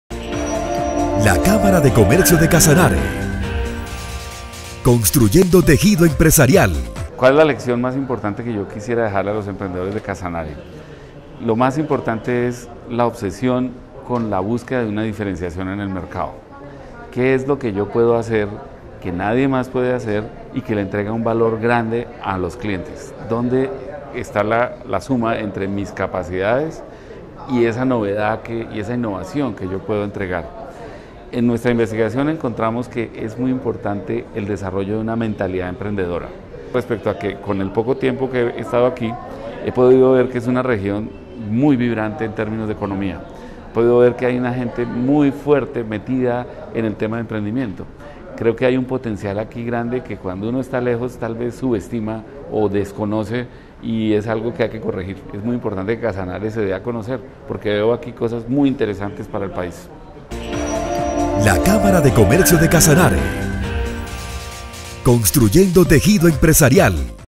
Rafael Vesga Fajardo - Conferencista Universidad de los Andes.
Docente de la Universidad de los Andes, que dictó capacitación de Emprendimiento e Innovación, que realizó la gerencia de Competitividad de la CCC, en la ciudad de Yopal.